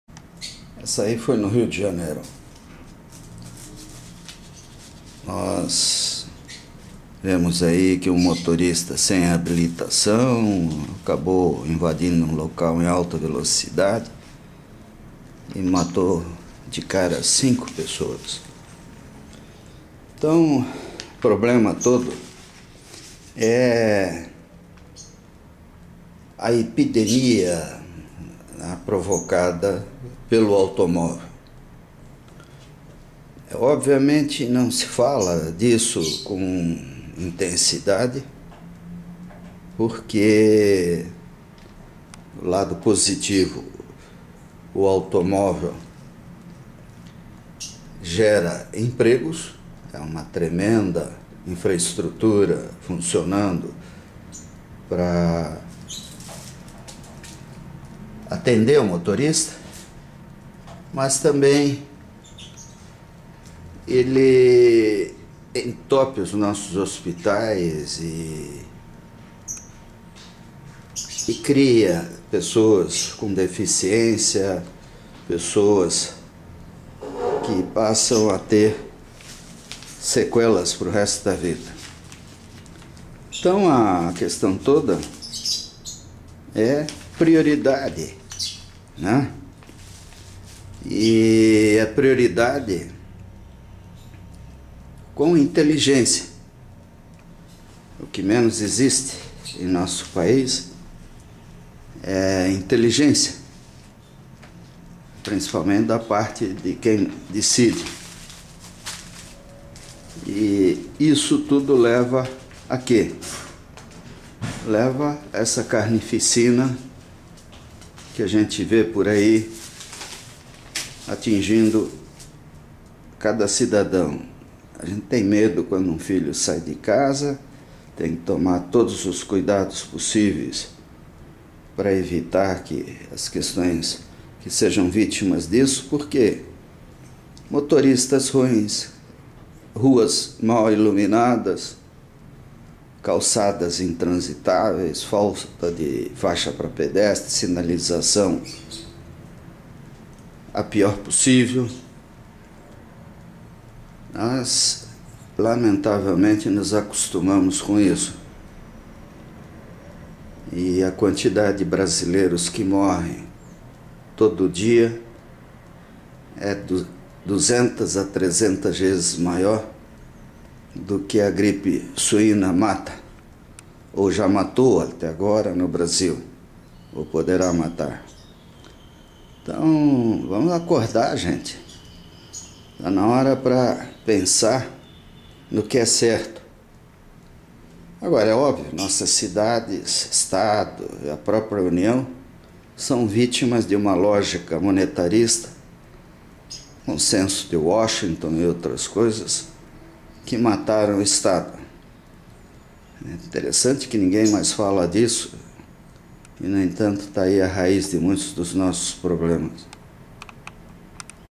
20090629201839 Atropelamentos - uma endemia evitável
A indústria do automóvel alijou ou degradou o transporte coletivo urbano de diversas formas e com muitas intenções.
A história é clássica.
O resultado é essa endemia, não é epidemia, de acidentes tenebrosos no Brasil.